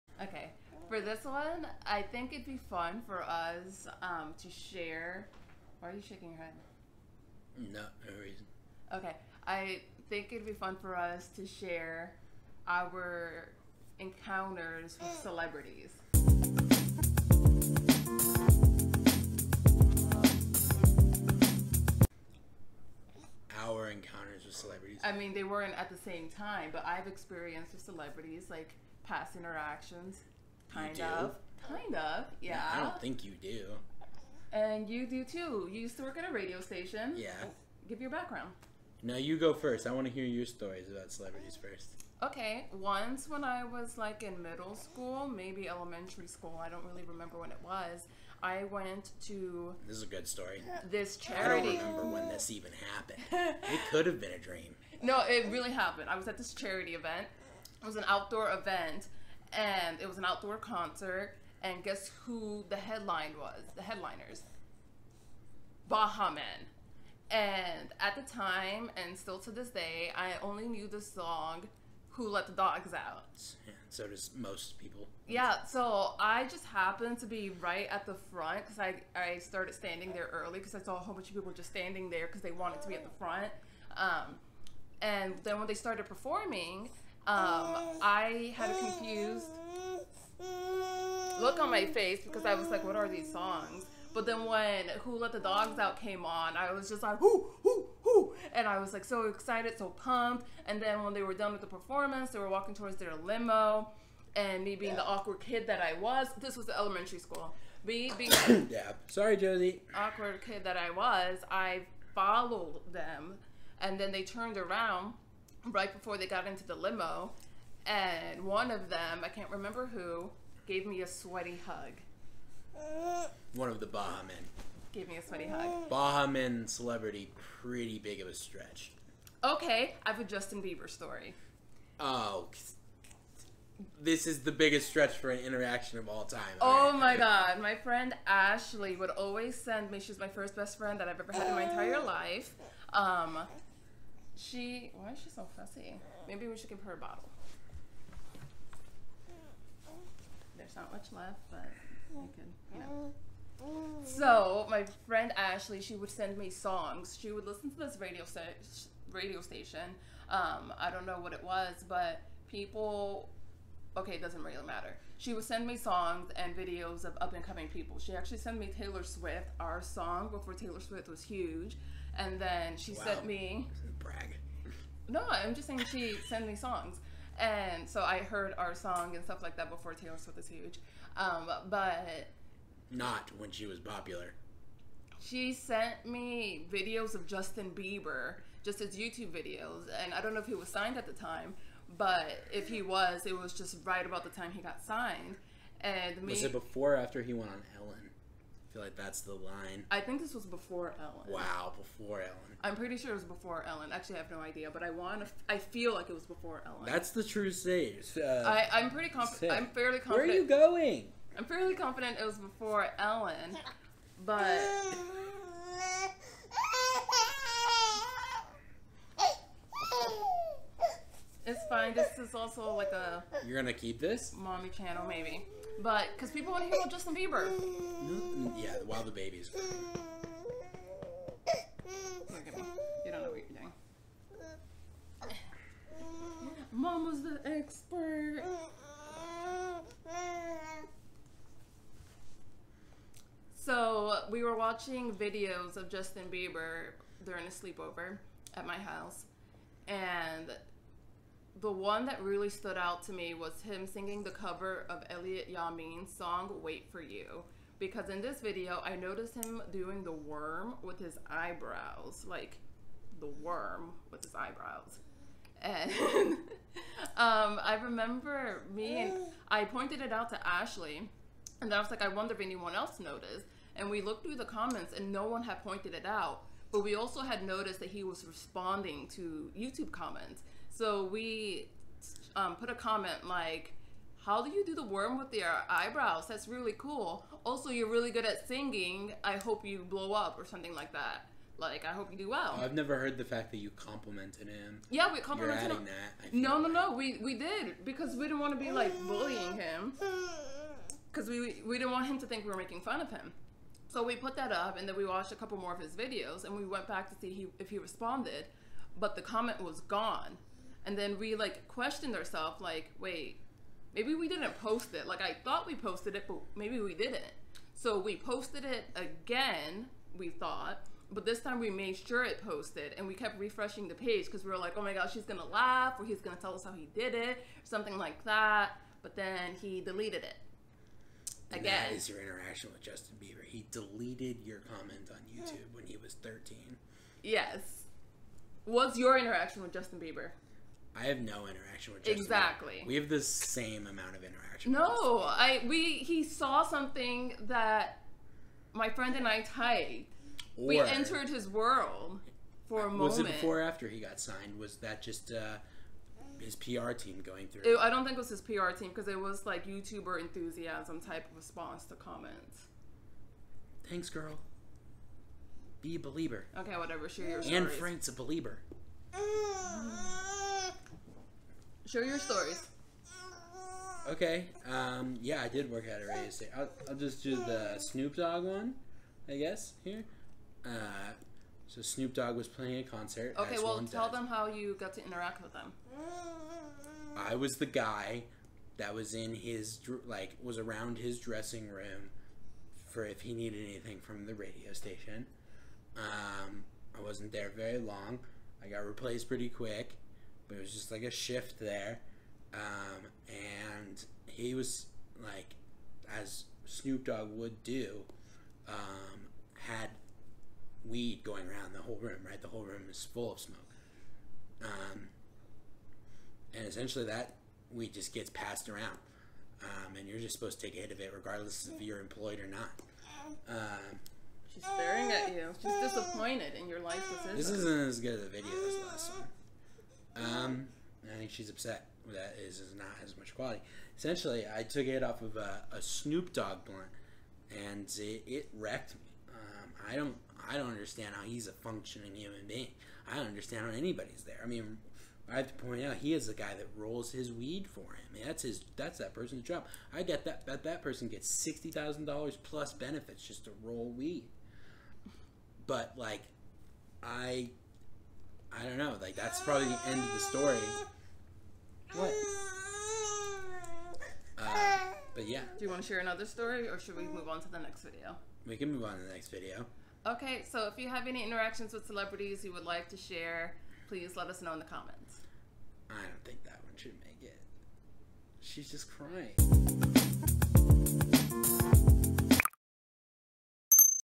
Storytime: Interactions With Celebrities (Baha Men, Justin Bieber, Snoop Dogg)
Join us for a celebrity-filled storytime! 🌟 In this video, Drew and I spill the tea on our past interactions with celebrities, including memorable encounters with the Baha Men, Justin Bieber, and Snoop Dogg. From unexpected run-ins to starstruck moments, we share our firsthand experiences and dish on the behind-the-scenes gossip.

So grab your popcorn and get ready for some laughs as we recount our brushes with fame! Whether you're a fan of celebrity gossip or just enjoy entertaining stories, this video is sure to keep you entertained from start to finish. 🎬

Channel Description
Just a weird girl diagnosed with complex post-traumatic stress disorder (cPTSD) and bipolar disorder learning to have an empowered life.